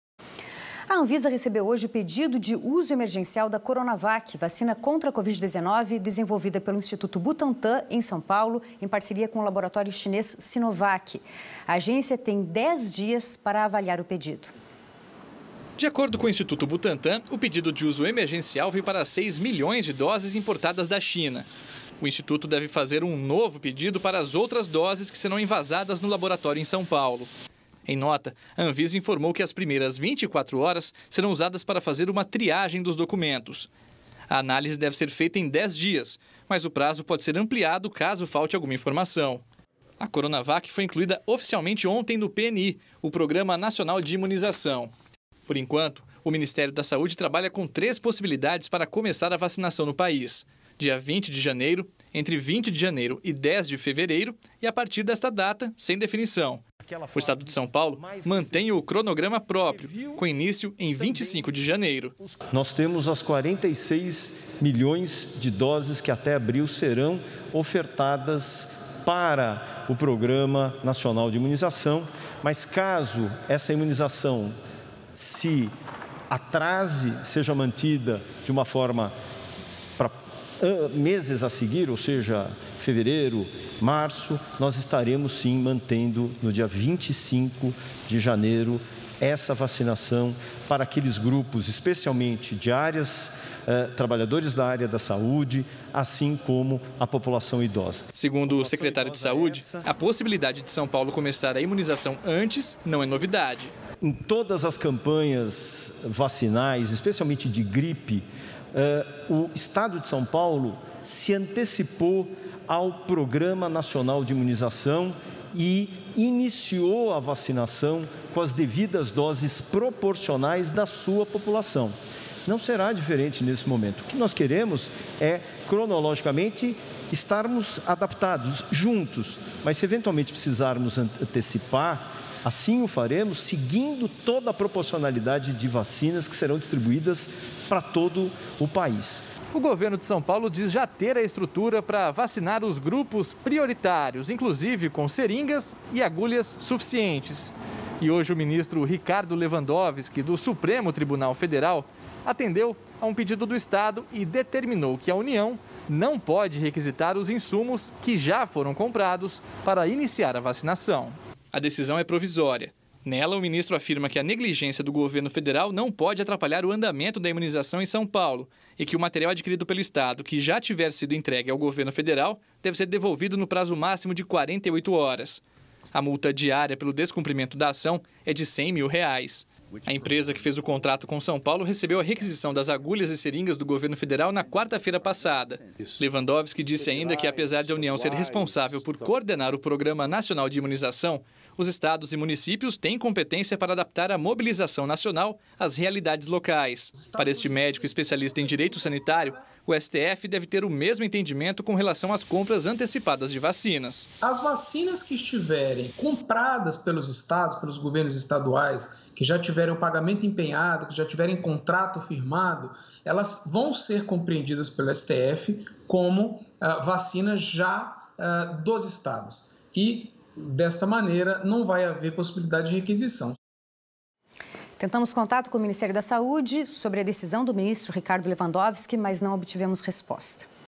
Anvisa recebeu pedido de uso emergencial da Coronavac
De acordo com o Instituto Butantan, o pedido de uso emergencial foi para as 6 milhões de doses importadas da China. O instituto deve fazer um novo pedido para as outras doses, que serão envasadas no laboratório em São Paulo.